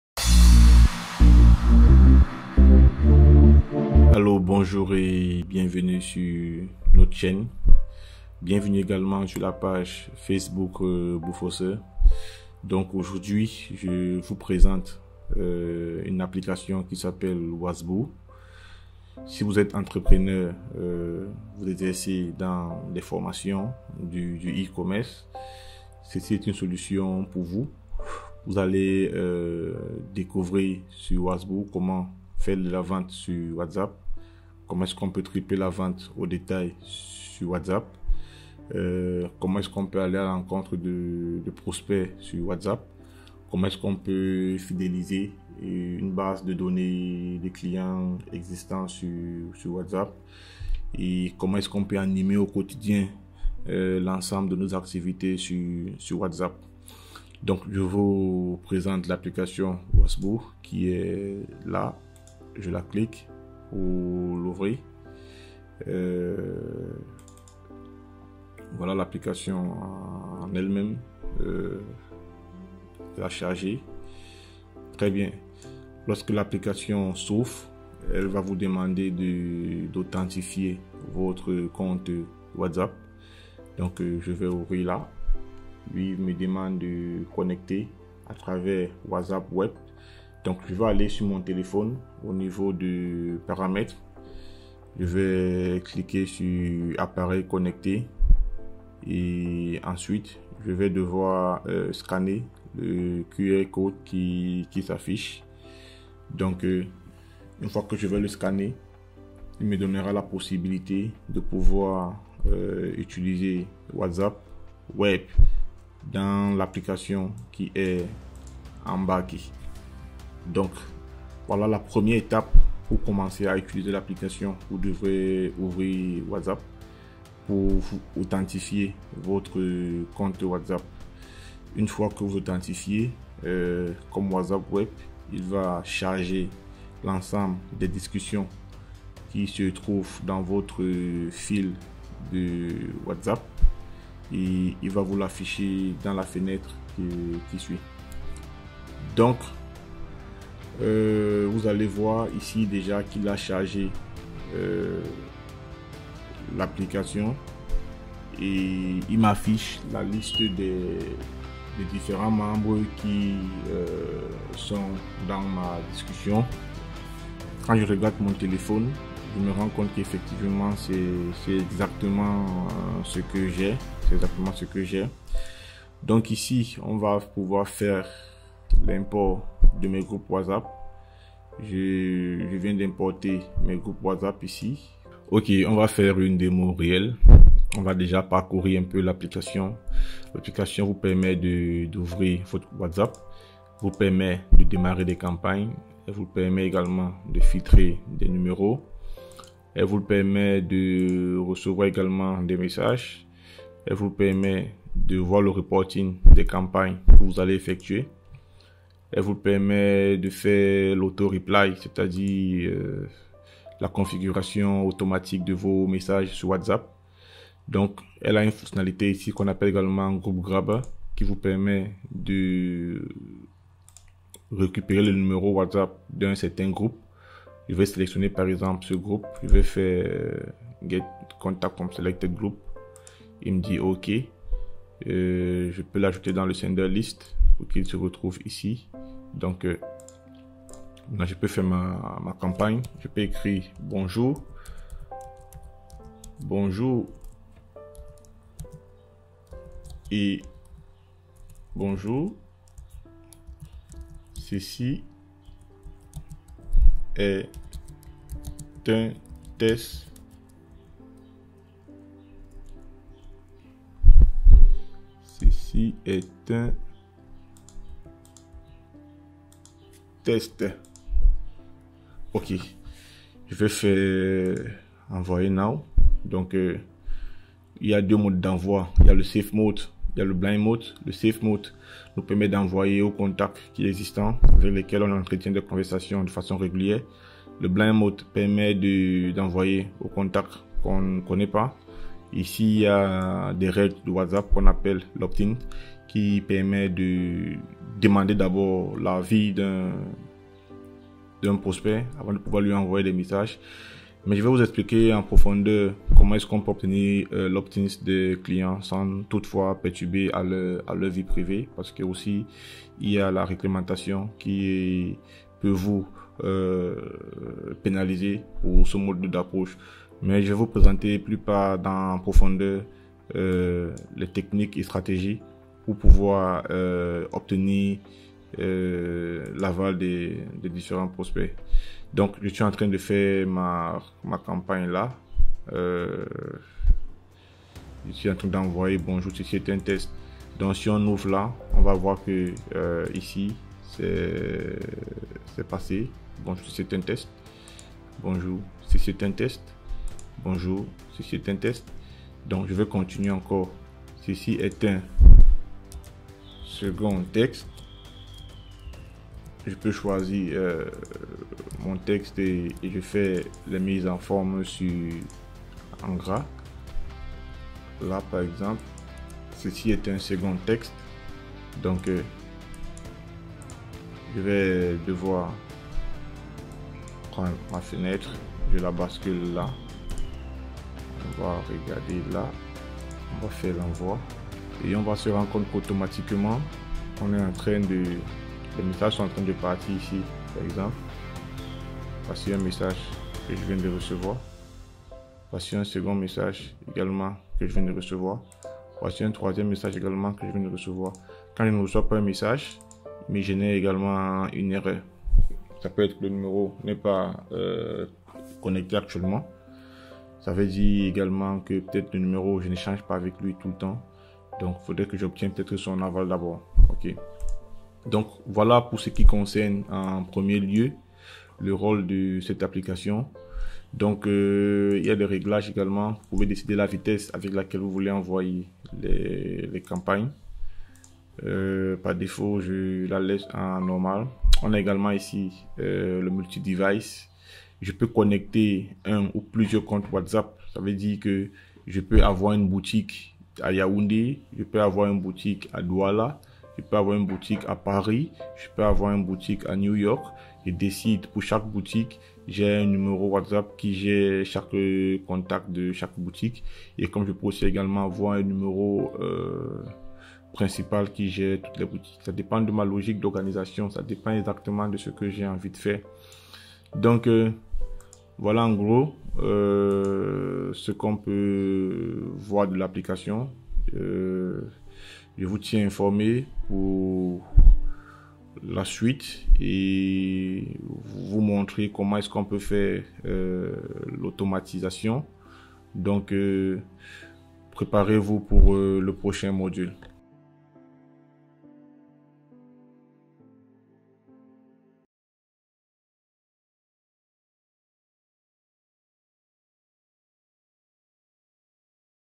Comment tripler la vente sur WhatsApp dès maintenant ?
Vous allez tripler les ventes dès maintenant.
Bonjour vous êtes entrepreneur , Coach, Formateur,E-commerçant ,vous souhaitez savoir comment animer la base de vos clients ,vous souhaitez savoir comment augmenter les ventes sur Facebook et WhatsApp alors cette vidéo t'intéresse

Ce que je vais vous apprendre

✅ Tripler la vente au Détail sur 𝗪𝗵𝗮𝘁𝘀𝗔𝗽𝗽
✅ Automatiser la relation client sur 𝗪𝗵𝗮𝘁𝘀𝗔𝗽𝗽 𝗕𝘂𝘀𝗶𝗻𝗲𝘀𝘀
✅ Envoyez des campagnes personnalisées sur 𝗪𝗵𝗮𝘁𝘀𝗔𝗽𝗽
✅ Découvrez Comment avoir plus de prospects sur 𝗪𝗵𝗮𝘁𝘀𝗔𝗽𝗽
✅ Plus de prospects + fidélisation + Gain de temps
✅ Stratégie de croissance